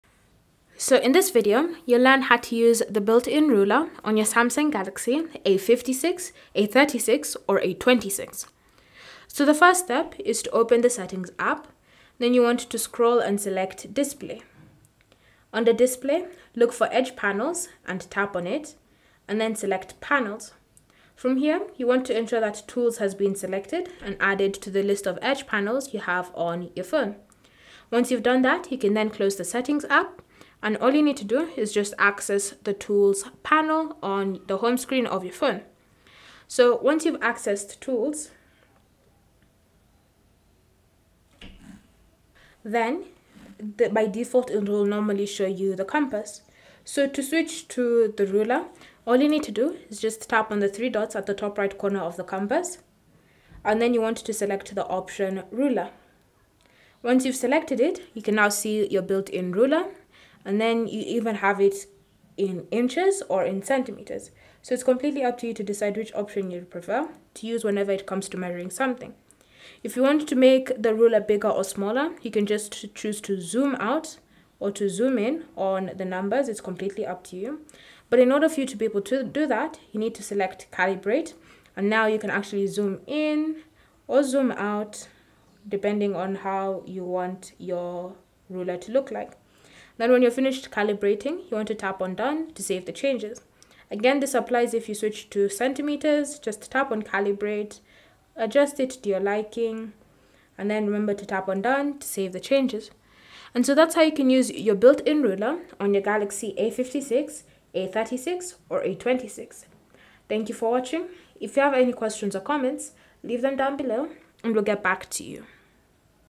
How To Use Built In Ruler On Samsung Galaxy A56/A36/A26
Learn how to use the built-in ruler on Samsung Galaxy A56/A36/A26 to measure objects directly on your screen with ease.

Whether you're working on a project or just curious about the size of something, the built-in ruler on Samsung Galaxy A56/A36/A26 can help you get quick and accurate measurements without needing an actual ruler.

-Open Settings.

-Scroll down and tap 'Display'.

-Select   'Edge panels'.

-Tap on  'Panels'.

-Ensure that Tools is selected as one of the edge panels.

-Exit Settings and go to the home screen.

-Swipe the Edge Panel to open the tools section.

-You will see the Compass by default.

-Tap the three dots at the top right corner of the compass.

-Select  'Ruler'  from the list.

-The ruler will now appear on the screen.

-Choose between inches or centimeters (cm).

-To adjust the size of the ruler, tap on  'Calibrate'.

-Use zoom in/out to resize the ruler to your preference.

-Once satisfied, tap  'Done'  to save your calibration.

Samsung Galaxy A56:

Samsung Galaxy A36:

Cell Phone Tripod Adapter